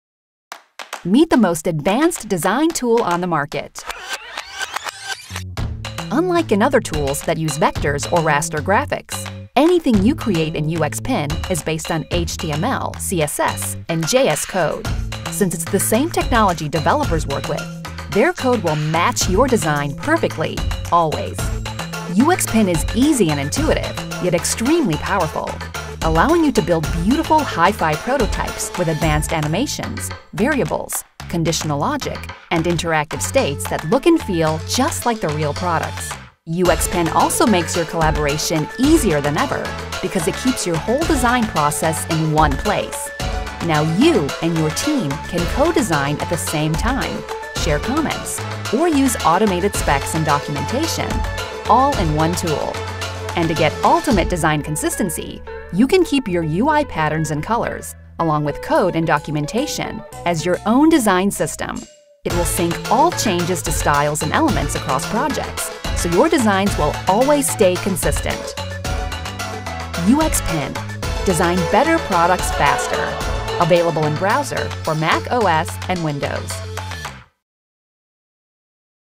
UXPin - UX Platform Unifying Design and Code
UXPin | The Premier UX Design Platform: The only end-to-end platform. Design systems, prototyping, and developer documentation tools. UXPin is the premiere UX design platform for interactive prototyping, design systems and documentation.

The platform currently supports the 5 parts of the design process in one place:

1. Design Systems – Create a design system of patterns, colors, text styles paired with metadata and code snippets.
2. Prototyping – Quickly design lo-fi and hi-fi prototypes across devices.
3. Collaboration – Gather feedback and iterate quickly.
4. Documentation – Document design decisions and use cases.
5. Developer Handoff – Automatic specifications, CSS, style guides for developers.